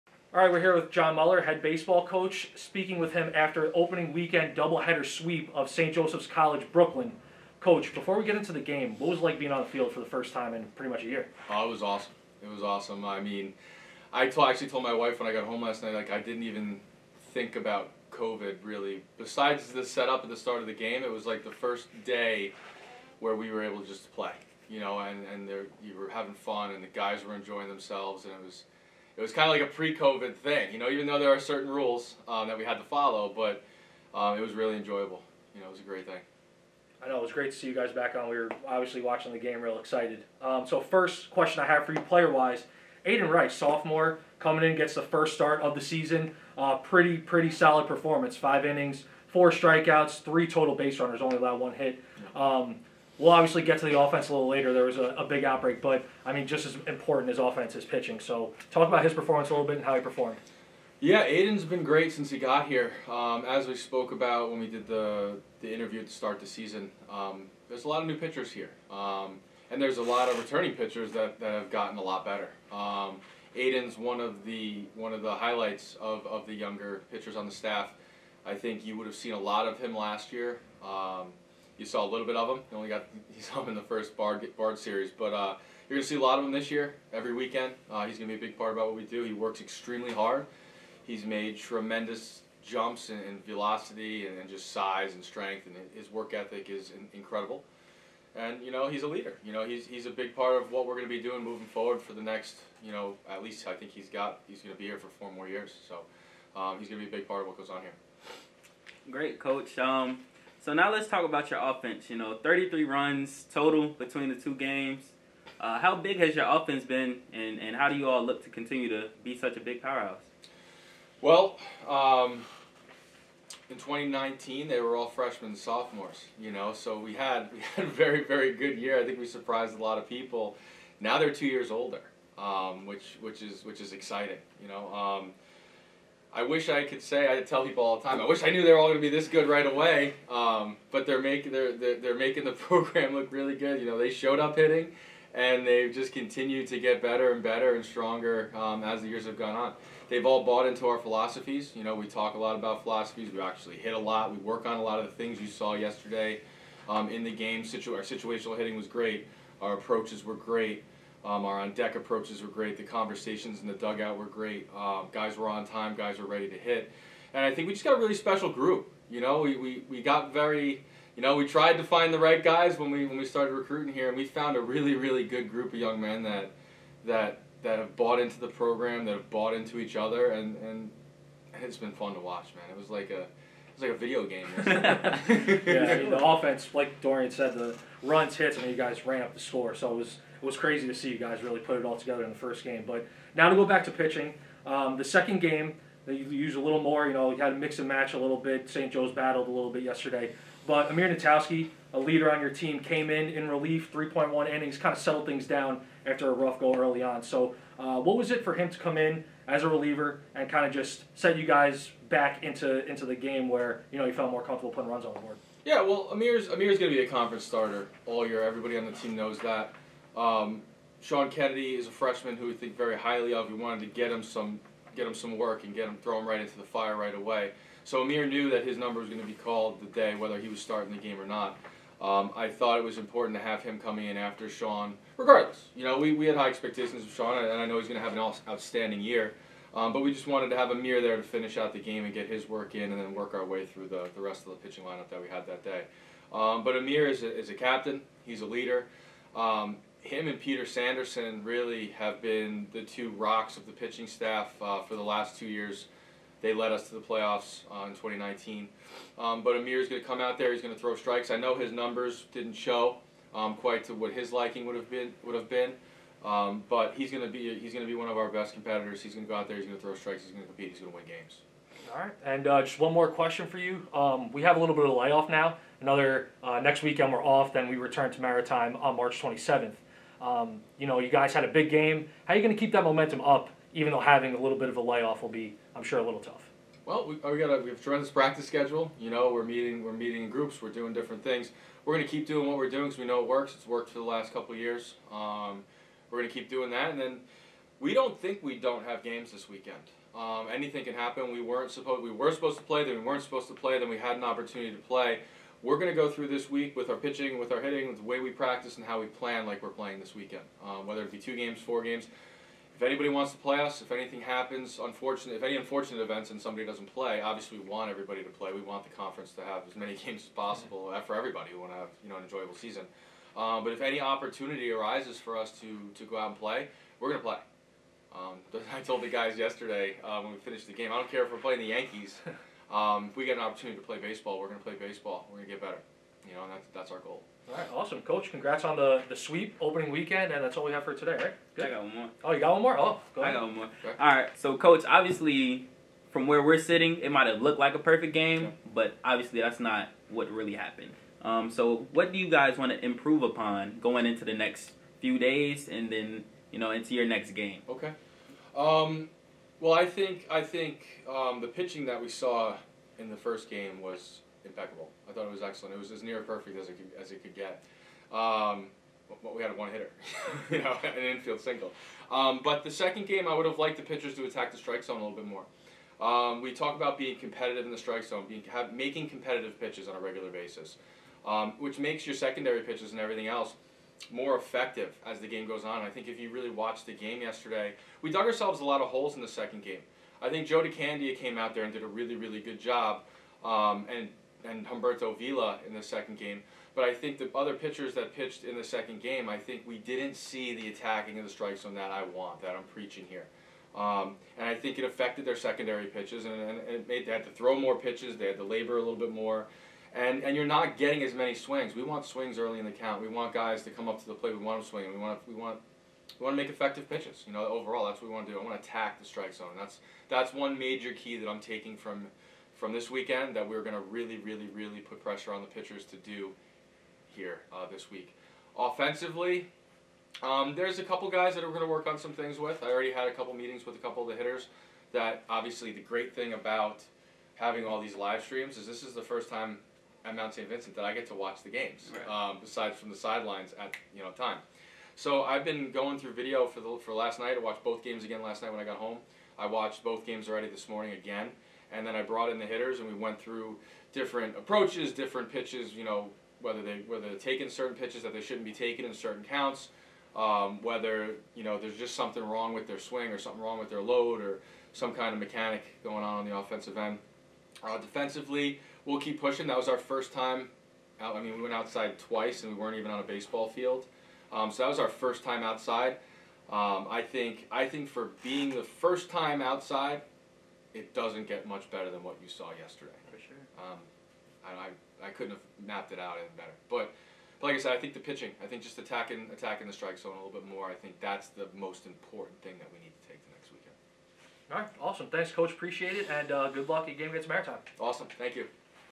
CMSV vs. St. Joe's Brooklyn Post Game Press Conference With John Muller
Baseball head coach John Muller sat with us to discuss his opening weekend double header sweep over the Bears, his  team's dominant offensive performance, breaking down his pitchers, the team's upcoming schedule and more.